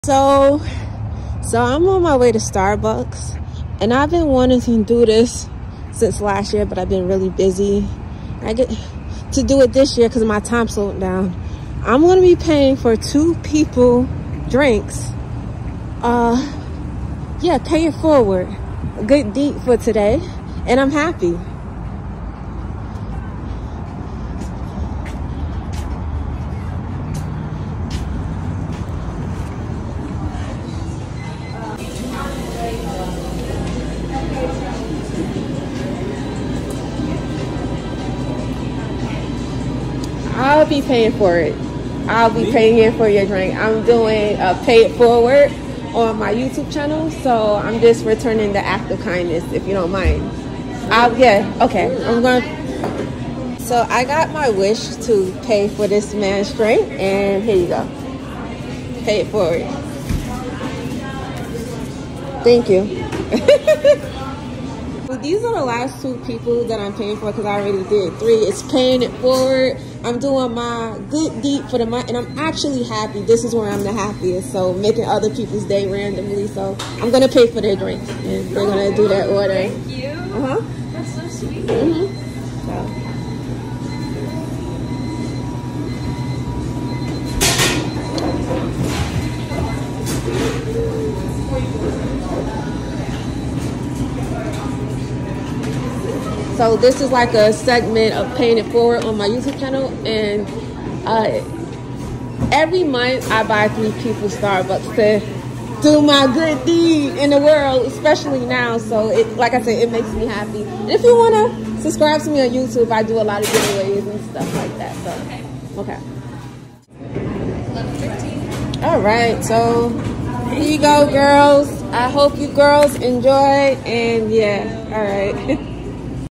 ☁️ EACH MONTH I TREAT 3 RANDOM STRANGERS TO : STARBUCKS coffee ☕️ I’m human first kindness is real
Hi guys each month at 3 random strangers to free coffee. Someone bought me free coffee yesterday and I didn’t get a chance to pay them back and I don’t think I will ever see that person again. She was a good friend of mine and a hard transition in my life ….. this is my way I’m paying it back to her but no really each month I’m treating three lucky people to STARBUCKS starbucks I know the world is dark we living in, but you still have good beings here, decent human beings here …… the small acts of gentle kindness is what I live for